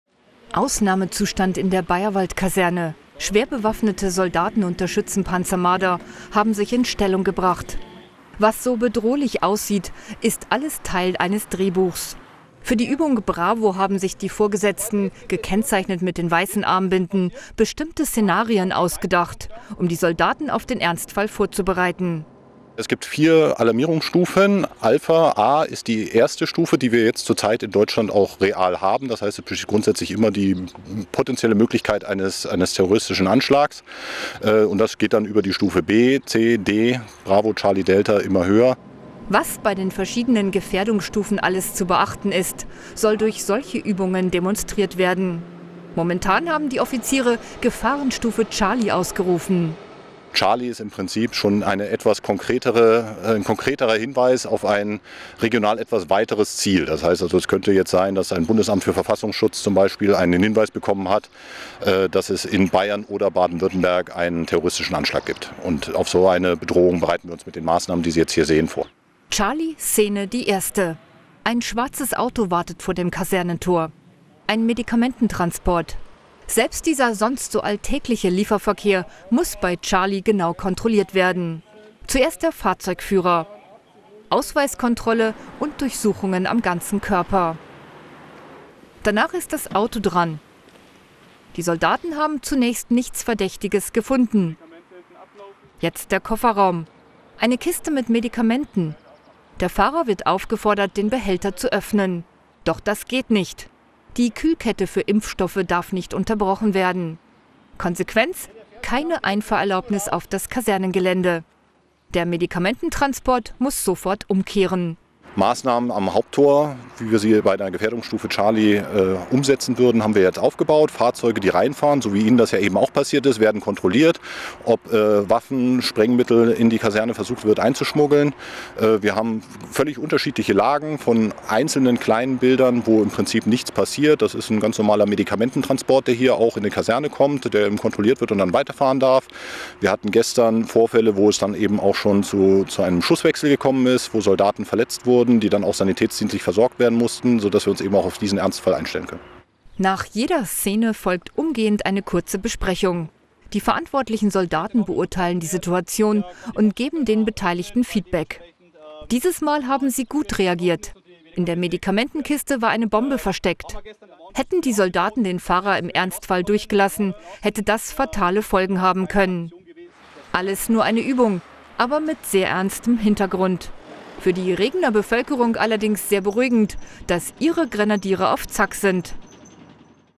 Terrorangriff – Regener Grenadiere üben Ernstfall vor der eigenen Haustür!
Sie sind im Kosovo und in Afghanistan im Einsatz und kennen die Risiken ihres Auftrags: Die Rede ist von den Regener Grenadieren. Doch „Sicherheit vor der eigenen Haustüre“ war bis dato nicht unbedingt ein Thema, mit dem sich die Bundeswehr speziell auseinandergesetzt hat. Durch die zahlreichen Terroranschlägen in Europa – und auch in Deutschland hat sich die Situation aber gravierend geändert. Und deshalb wurde nun auch beim Panzergrenadierbataillon 112 in Regen der Ernstfall geprobt.